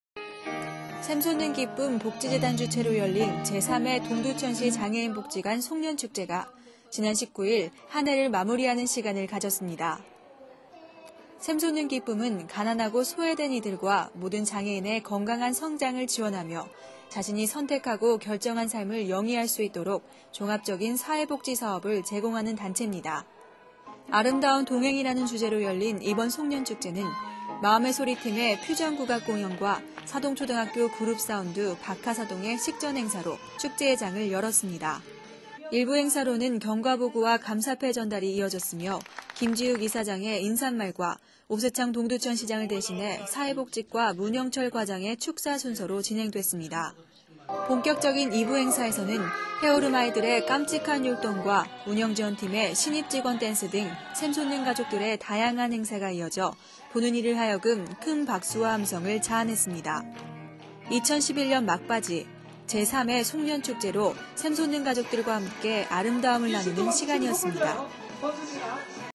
제3회 동두천시 장애인복지관 ‘송년축제’ 2011 12 19
샘솟는기쁨 복지재단 주최로 열린 제 3회 동두천시 장애인복지관 ‘송년축제’가 지난 19일 한 해를 마무리하는 시간을 가졌습니다. 샘솟는기쁨은 가난하고 소외된 이들과 모든 장애인의 건강한 성장을 지원하며 자신이 선택하고 결정한 삶을 영위할 수 있도록 종합적인 사회복지사업을 제공하는 단체입니다. 아름다운 동행이라는 주제로 열린 이번 송년축제는 마음의소리 팀의 퓨전국악공연과 사동초등학교 그룹사운드 박하사동의 식전행사로 축제의 장을 열었습니다. 1부 행사로는 경과보고와 감사패전달이 이어졌으며, 김지욱 이사장의 인사말과 오세창 동두천 시장을 대신해 사회복지과 문영철 과장의 축사 순서로 진행됐습니다. 본격적인 2부 행사에서는 해오름 아이들의 깜찍한 율동과 운영지원팀의 신입직원 댄스 등 샘솟는 가족들의 다양한 행사가 이어져 보는 이를 하여금 큰 박수와 함성을 자아냈습니다. 2011년 막바지, 제 3회 송년축제로 샘솟는 가족들과 함께 아름다움을 나누는 시간이었습니다.